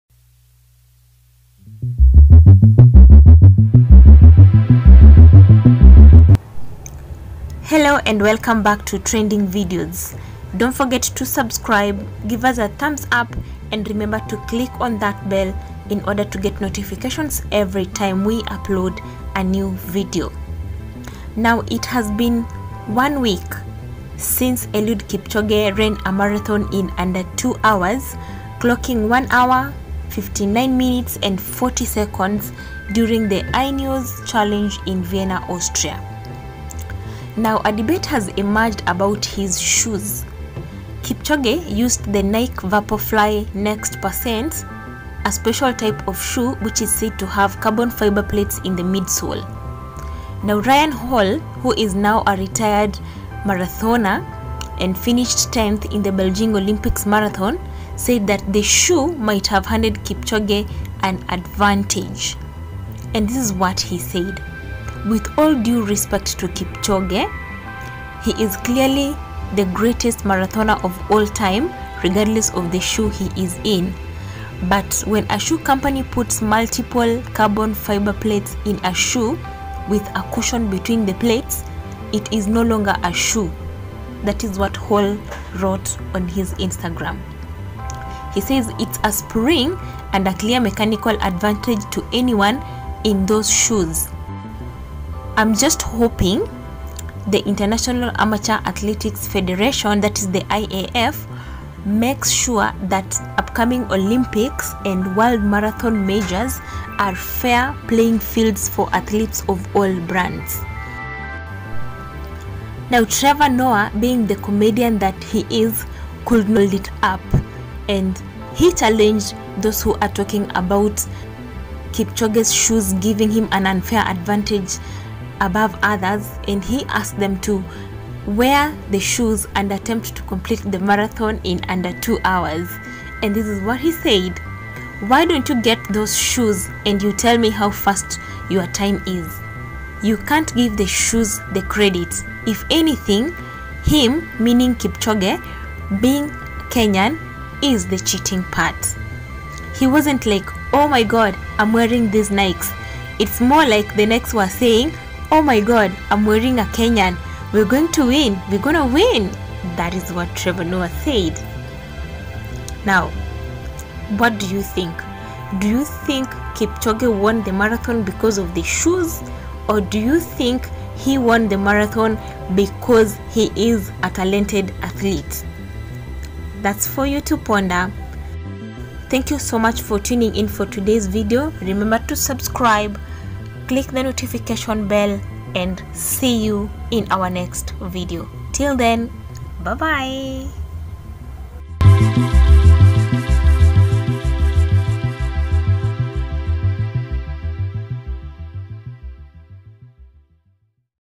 Trevor Noah's message to Those Who Think Eliud Kipchoge Won Because of The Nike Shoes
TrevorNoah dishes out a hilarious response to those who think EliudKipchoge won INEOS1.59challenge in Vienna Austria because of the Nike VaporflyNext%